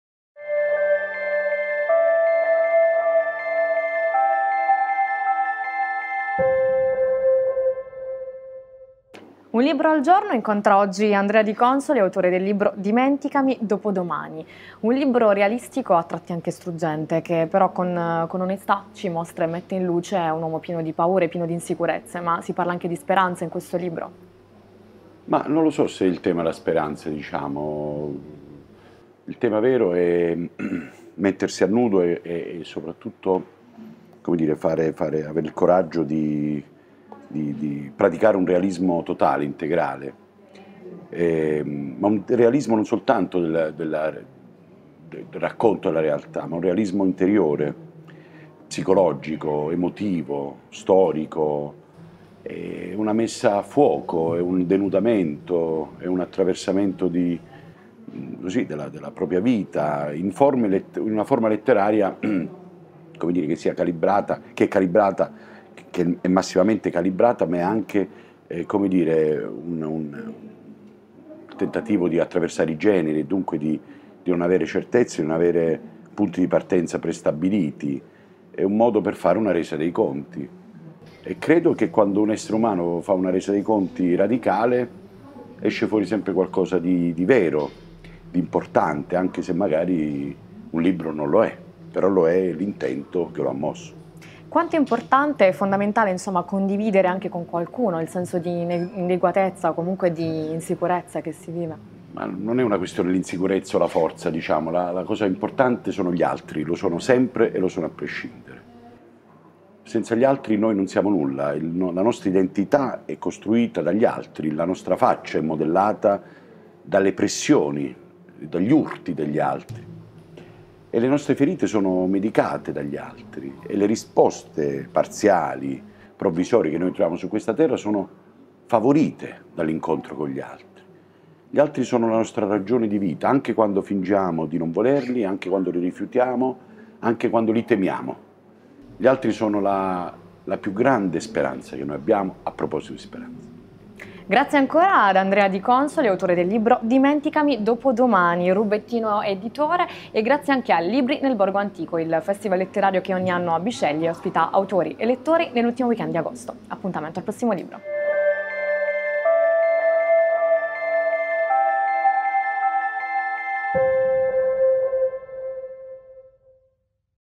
"Libri nel Borgo antico". Presentato “Dimenticami dopodomani”
“Dimenticami dopodomani” di Andrea Di Consoli è tra i libri presentati a "Libri nel borgo antico", a Bisceglie. Una riflessione su disincanti e fallimenti della generazione di mezzo.